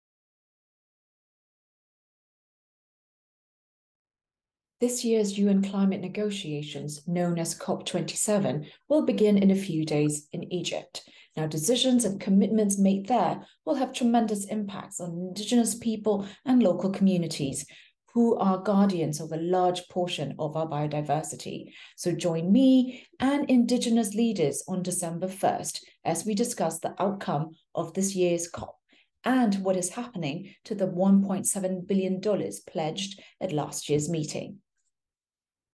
Thin Lei Win- Post COP27: Reflecting on Donor Promises to Forest Guardians
Thin Lei Win, Climate and Food Correspondent, gives us a sneak peek into the upcoming Land Dialogue on Post COP27: Reflecting on Donor Promises to Forest Guardians.